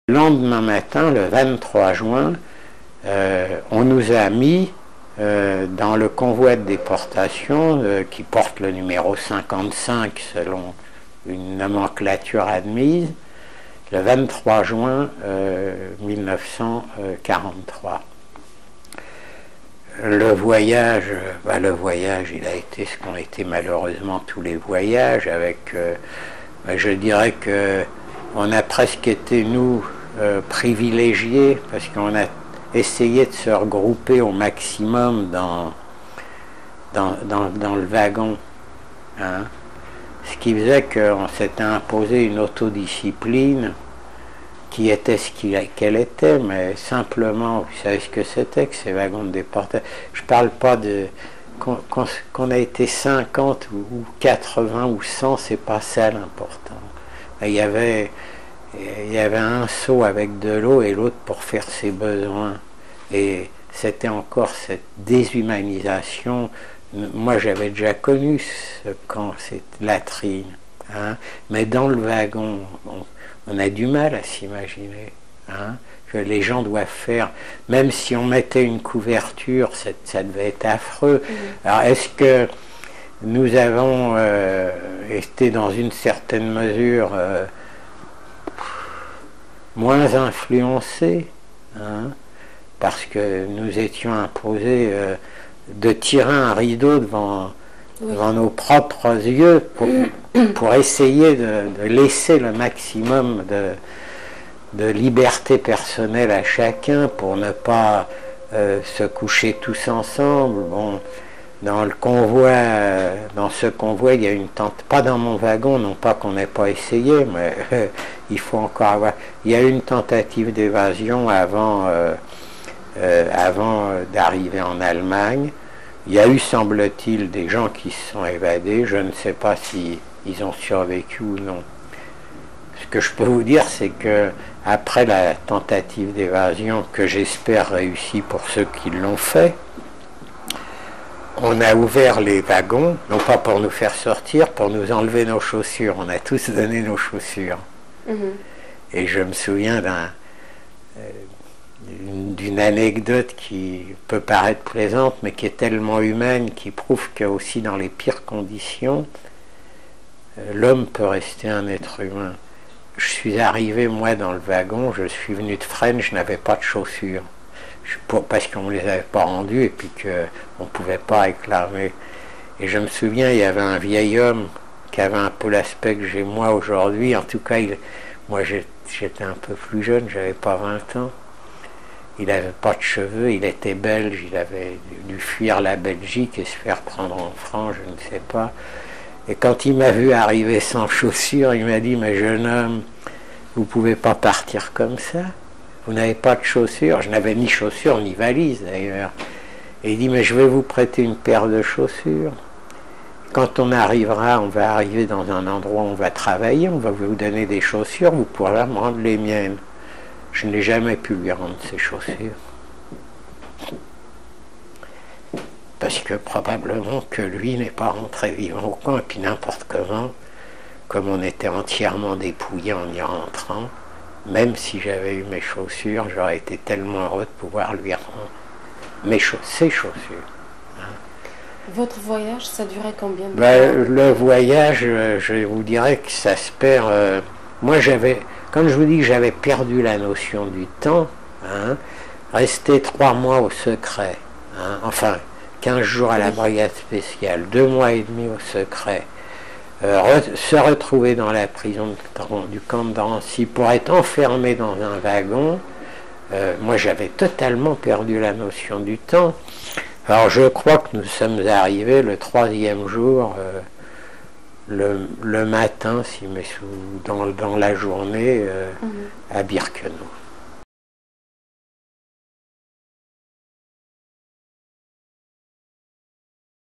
Samuel Radzynski - déporté de Drancy à Auschwitz le 23/06/1943
Extrait du témoignage de Samuel Radzynski, décrivant sa déportation de Drancy, les conditions du voyage en train et l'arrivée à Auschwitz en juin 1943. Entretien réalisé par l'USC Shoah Foundation Institute.